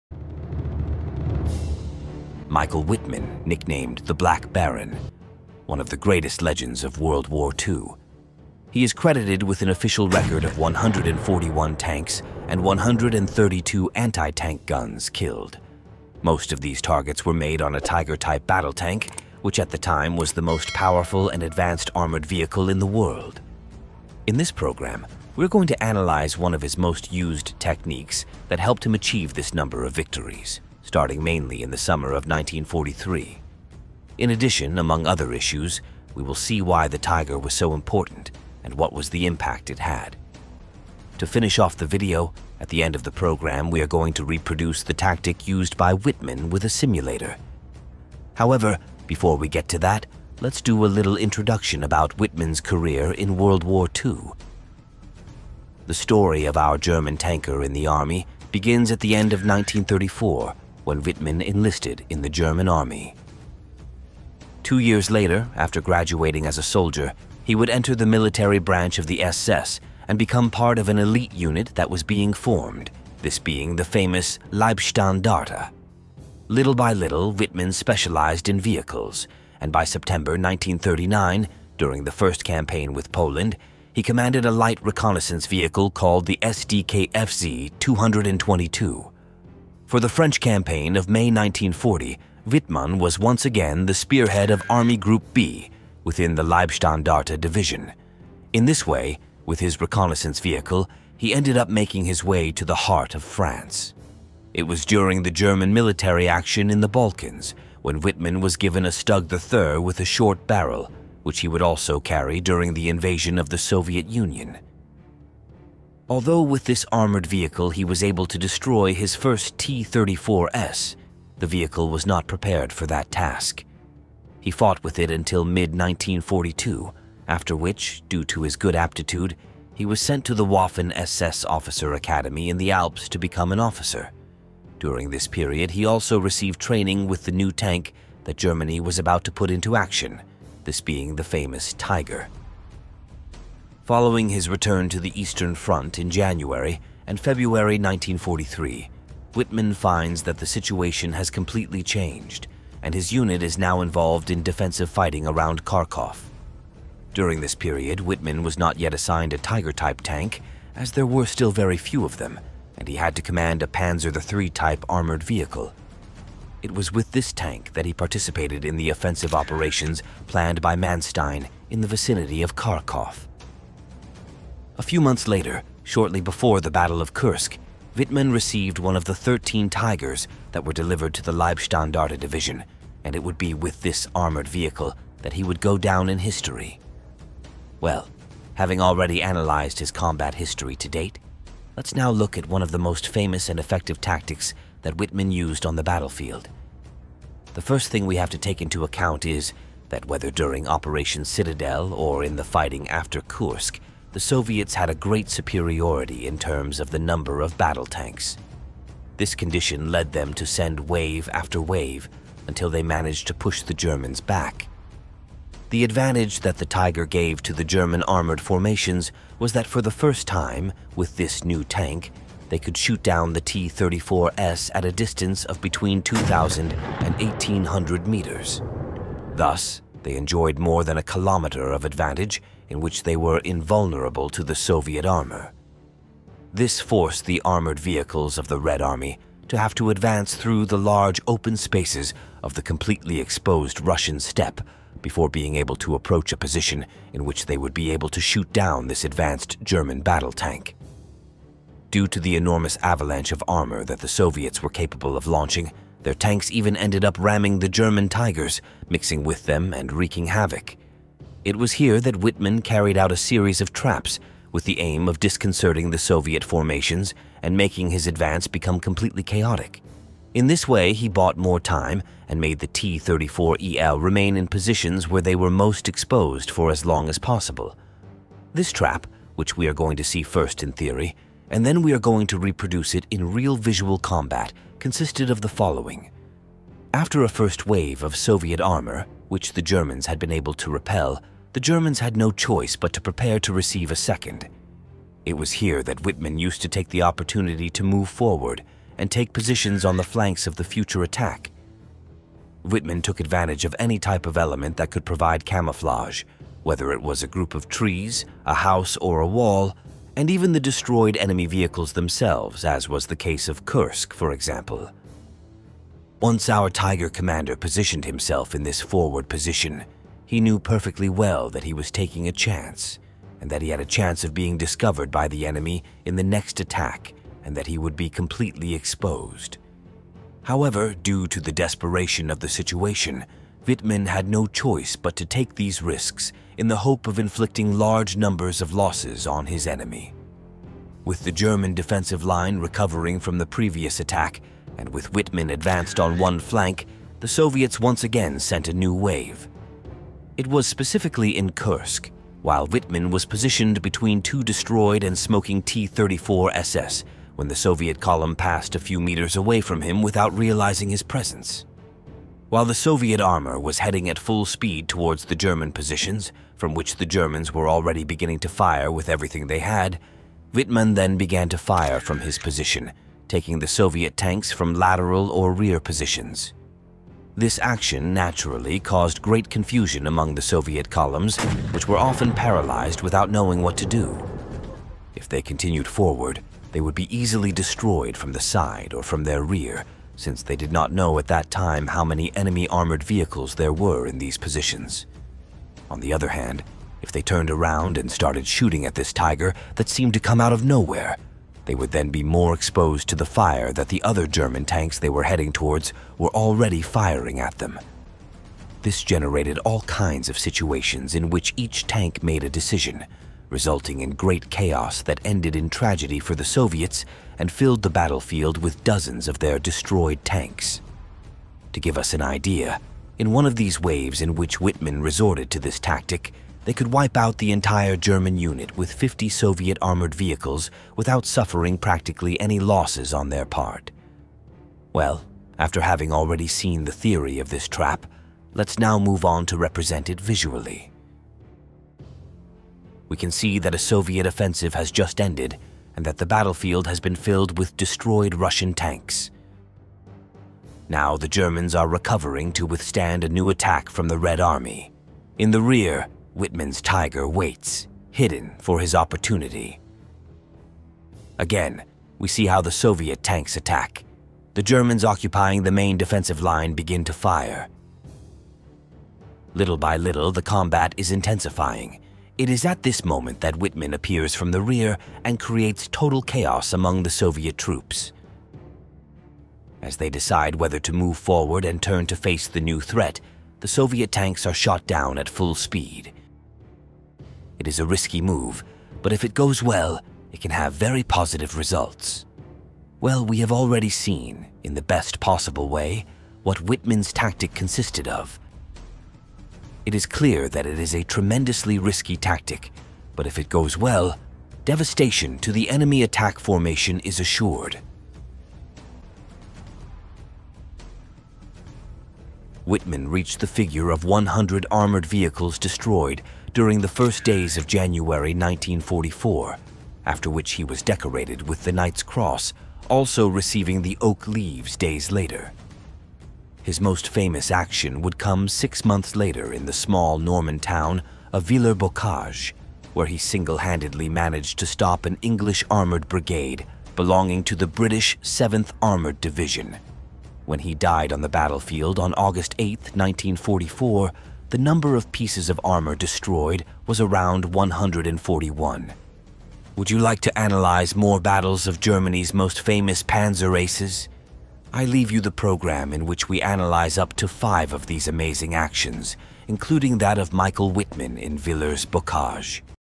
Panzer Ace Michael Wittmann's Favorite Trap | Tactics of the German Black Baron
What was Wittmann's career like within the German Army? What new features did the Tiger introduce when it entered combat? What was Wittmann's favorite tactic for hunting Soviet tanks? Why was it so risky? What was the death of Michael Wittmann like?

 Next, in this program we analyze it!

00:00 Michael Wittmann: Early years
02:00 Wittmann receives the Tiger I
03:00 Advantages Tiger I
04:23 Wittmann trap
07:23 Simulated representation
08:32 The end of Wittmann